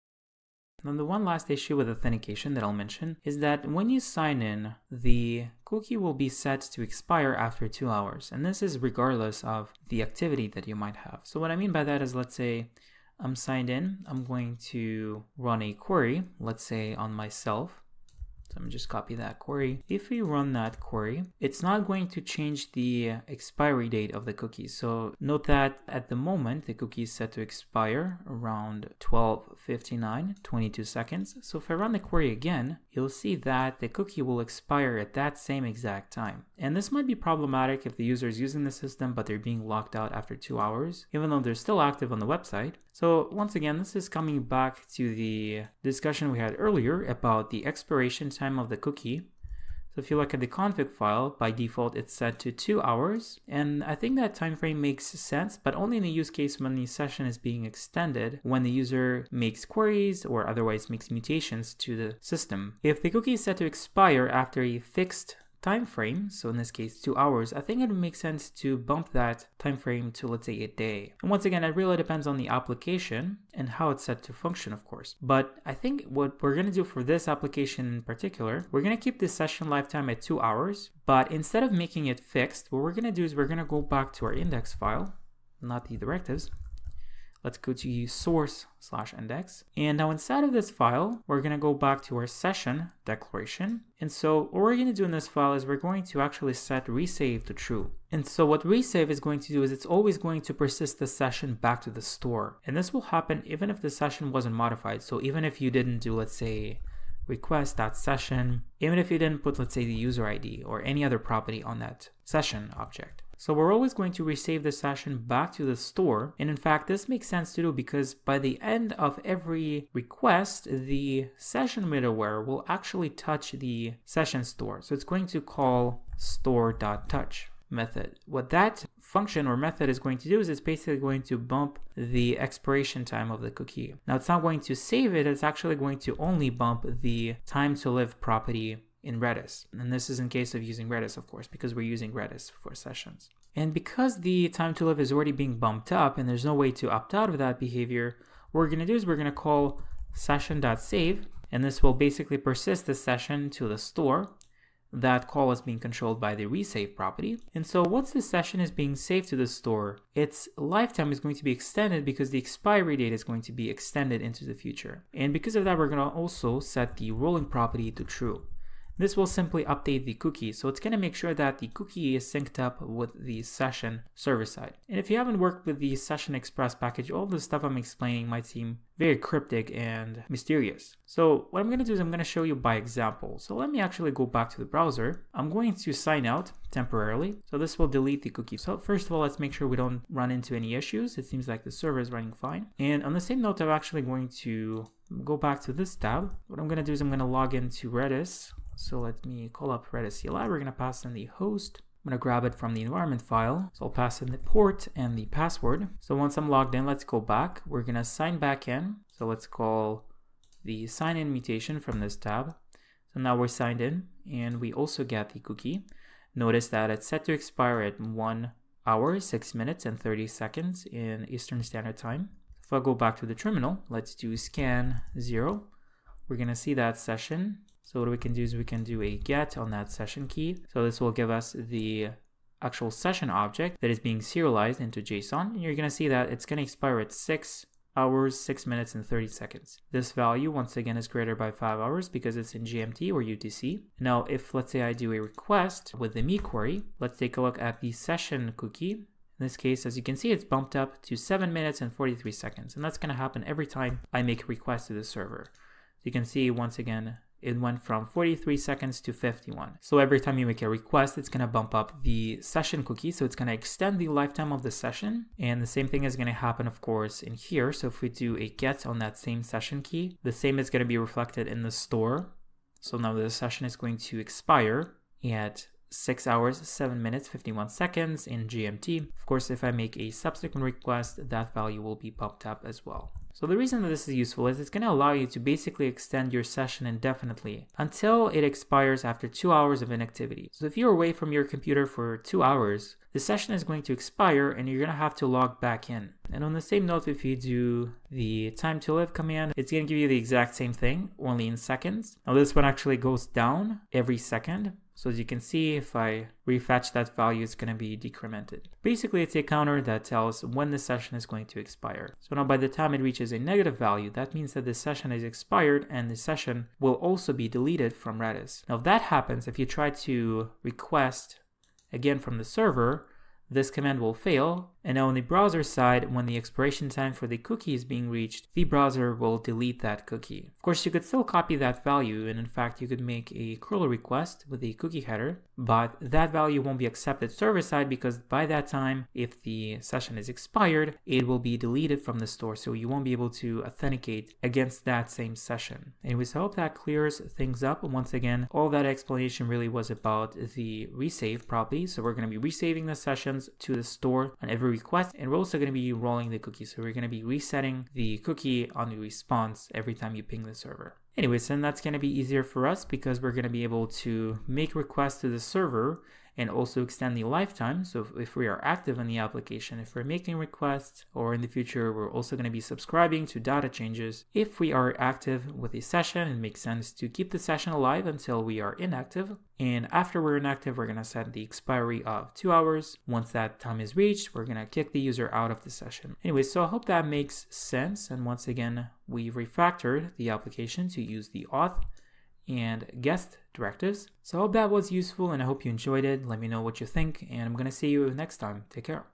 MERN Stack & GraphQL - #12 Session Lifetime
If this video didn't make sense: express-session always calls session.touch() on every request. This does two things: (1) resets session.expires date to keep it alive (but does NOT actually save it to Redis), and (2) resets TTL (time-to-live) in Redis to its original cookie.maxAge ÷ 1000 (in seconds). Why is this a problem? Because it leaves session.expires out of sync with TTL, and opens a backdoor for infinite sessions. There is no way to opt out of this! But we can work around it.

What did we do? We set "resave: true" so that when its expiry date is reset, the session is re-saved to the store to reflect that change, and also to stay in sync with TTL. We also set "rolling: true" to send a new cookie should the expiry date change. This way, we keep all three (the expiry date, the TTL, and the cookie) in sync. Your session stays alive as long as you keep using the site, but after you leave for 2+ hours, you have to log back in. The 2 hours could be 12, 24, etc. hours at your discretion. IMO that's the most reasonable approach given the constraints.